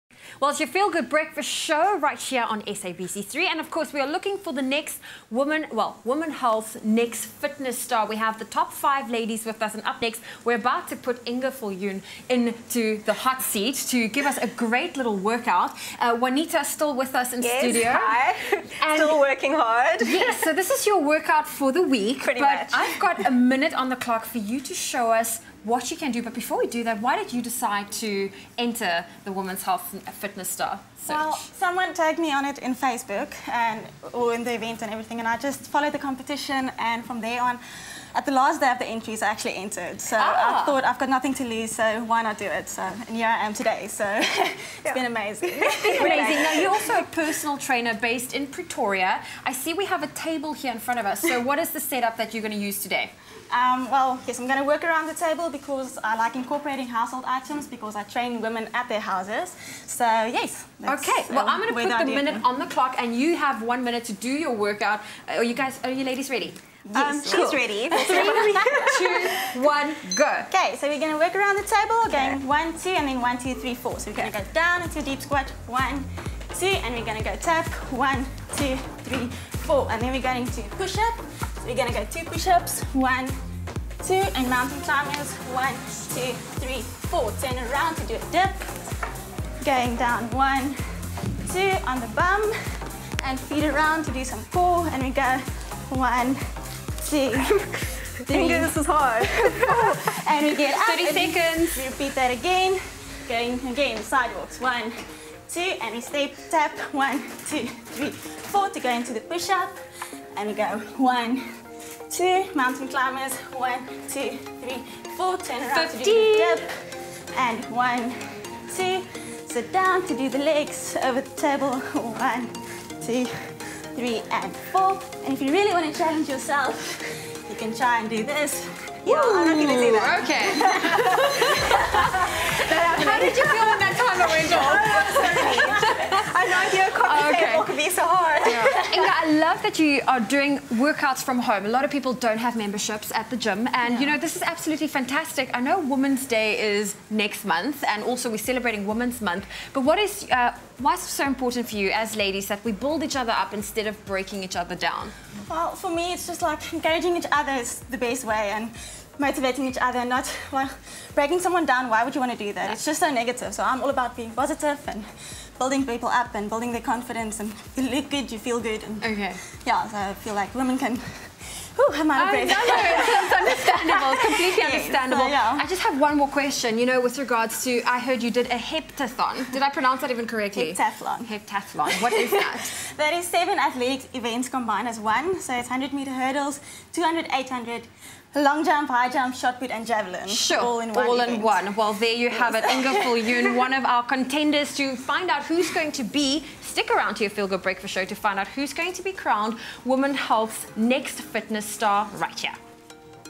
Women’s Health Next Fitness Star Finalist 2/5 - Inge Viljoen | 24 July 2017
Today we have a Women’s Health Fitness Star take over and the end of the show we will announce which one of our 5 finalists will be walking away with as the next fit star. Each person will get 1 minute to put the group through a fun and out of the box fitness routine and tell us why they entered.